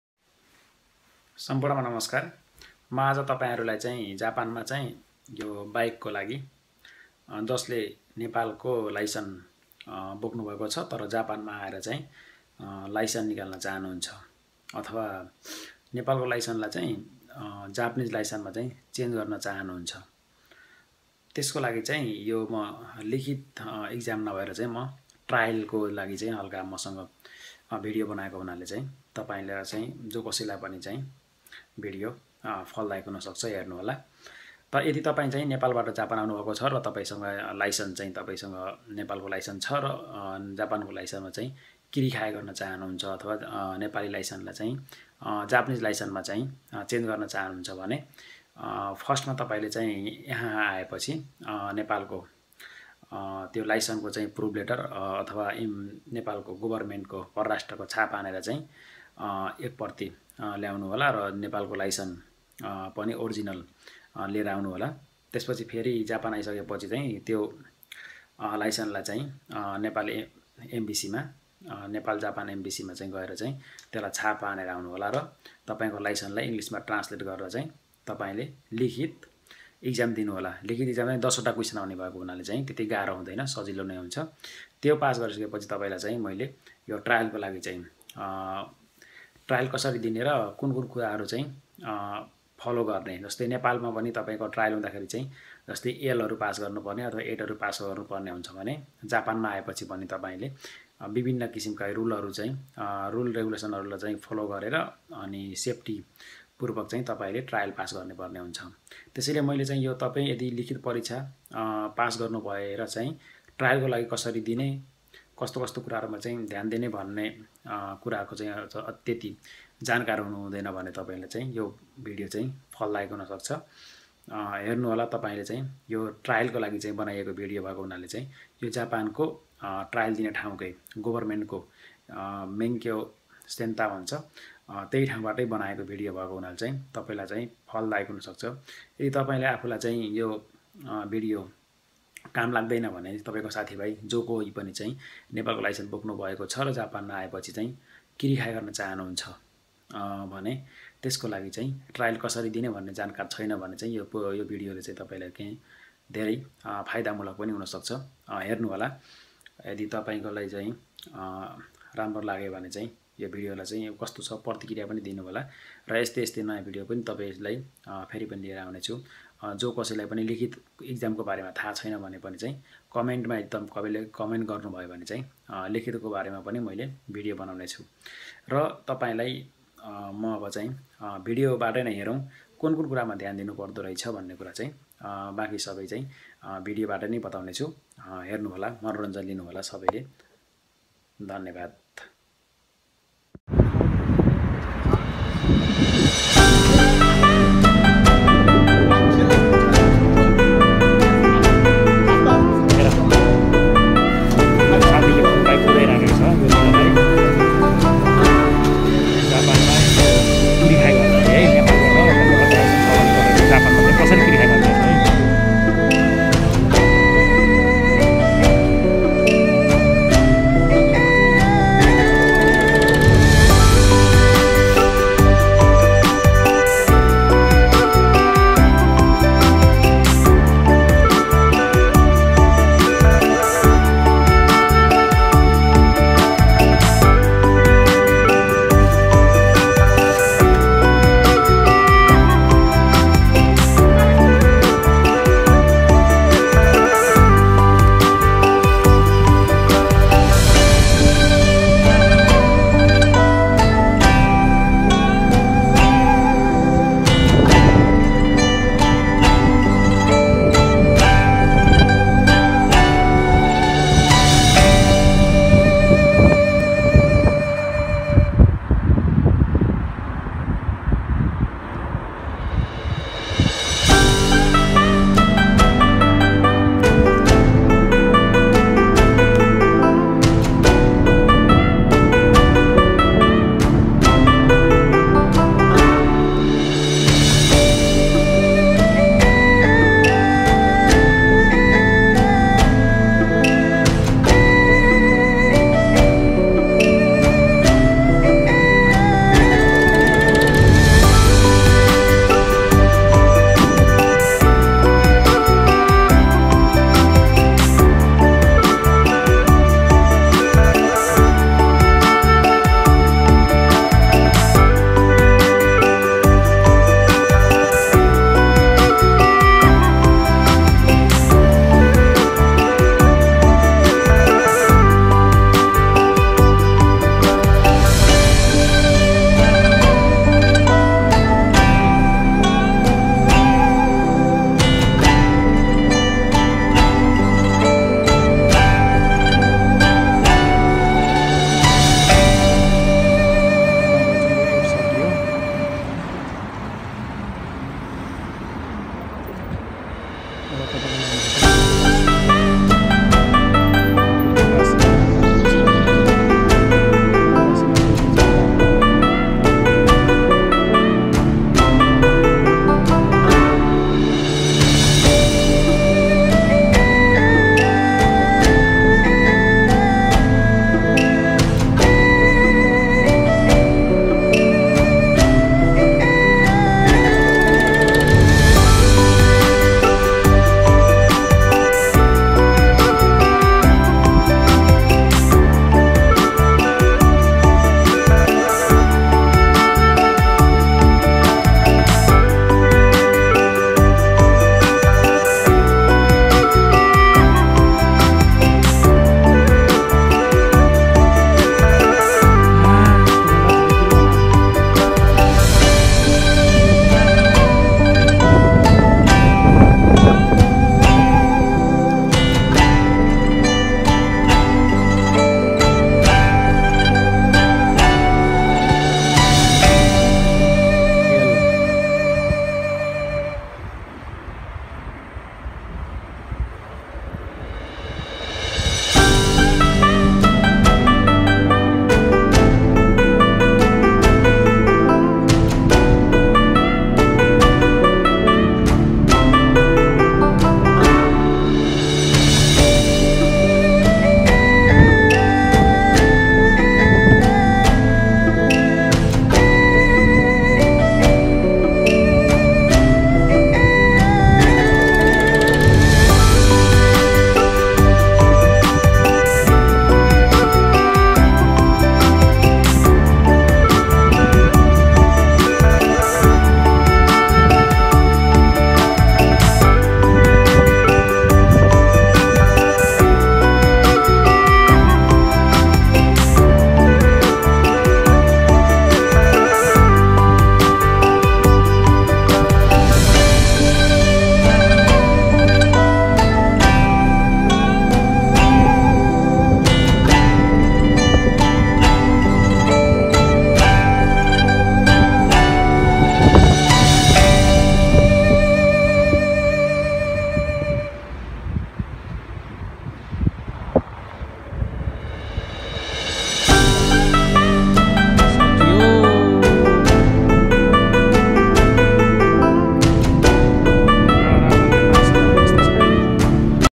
licence# नेपालि बाइक को लाइसेन्स लाई जापान मा कसरि ट्रायल पास गर्ने
Japan ma bike license kasari convert garne

Nepalitips4U